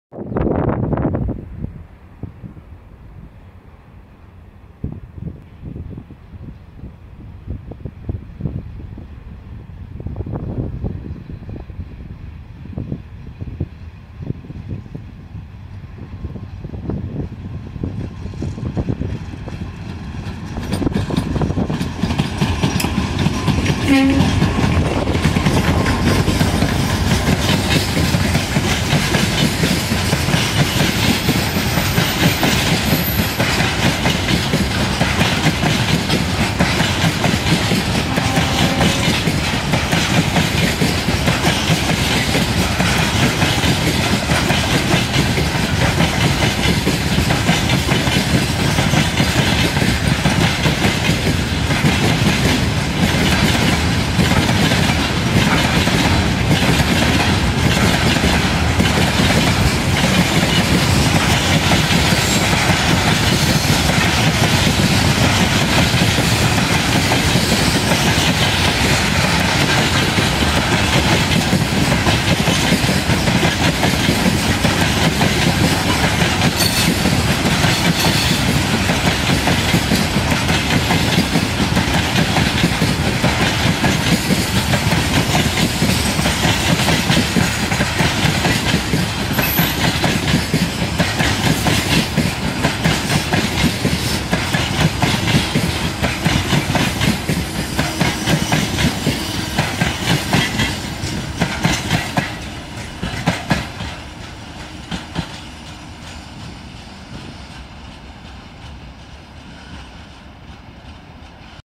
2ТЭ10M - 2417 с грузовым поездом ( приветливая лок.бригада )
Одесская железная дорога
Перегон: Николаев пасс. - Николаев груз.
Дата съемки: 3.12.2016
Снято на: HTC One_M8